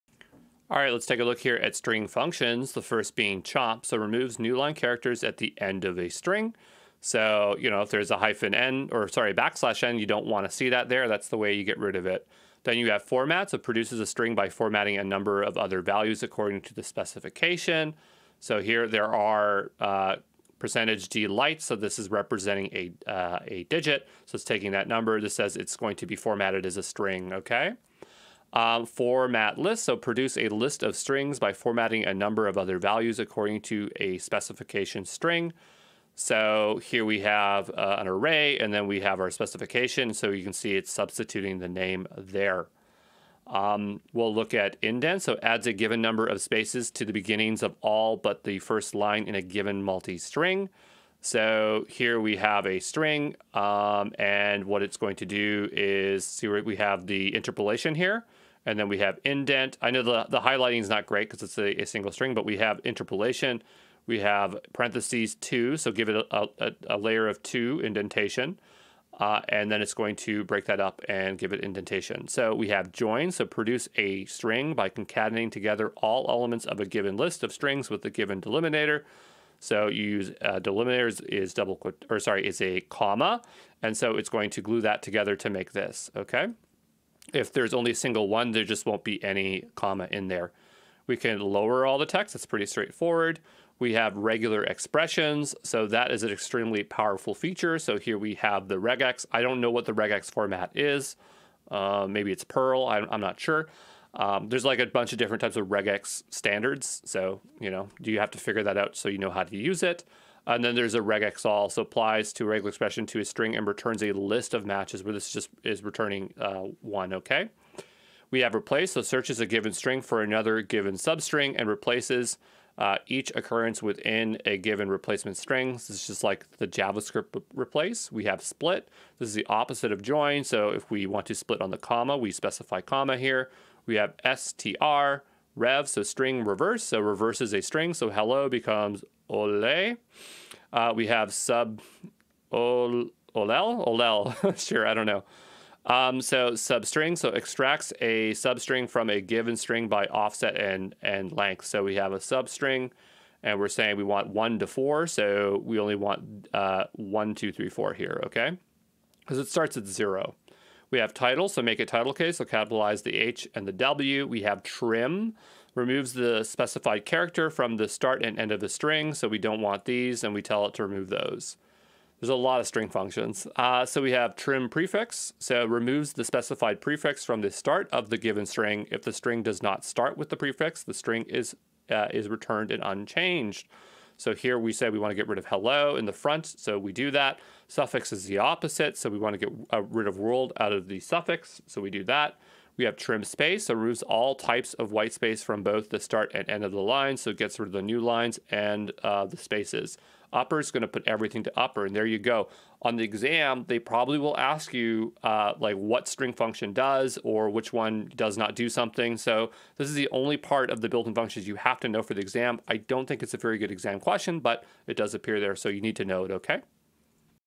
String Functions in Terraform - HashiCorp Terraform Associate (003)
Learn about string functions in Terraform, including operations like splitting, joining, and formatting strings. Discover how these functions help you manipulate text data effectively.